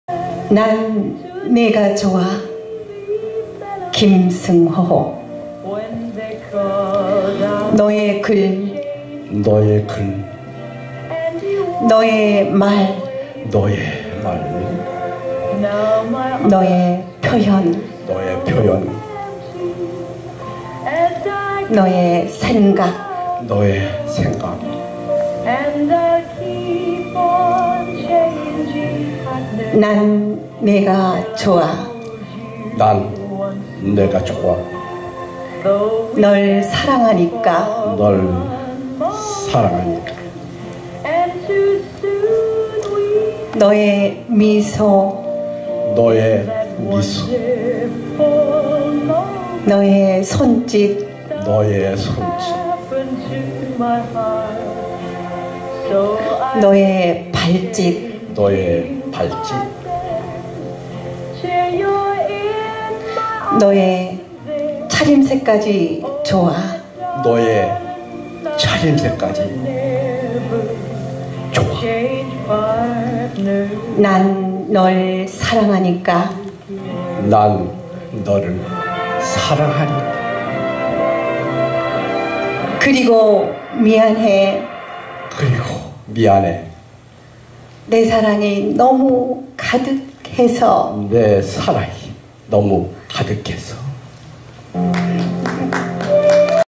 [댄스스포츠 & 시낭송] 난 네가 좋아(김승호 시)...이상연 교수, 안종숙 사회복지사 / 2018년 한국다선문인협회 제3회 불우이웃돕기 시낭송회 제1부
2018년 한국다선문인협회 제3회 불우이웃돕기 시낭송회
"사랑과 나눔 그리고 더 큰 빛"
사회 : 조기홍 수석감사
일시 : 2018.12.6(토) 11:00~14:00
장소 : 고양시 덕양구청 대강당
주관 : (사)한국다선예술인협회,
           한국법무보호복지공단 사회성향상위원회
주최 : (사)한국다선문인협회, (사)한국다선문학작가회, 신문고뉴스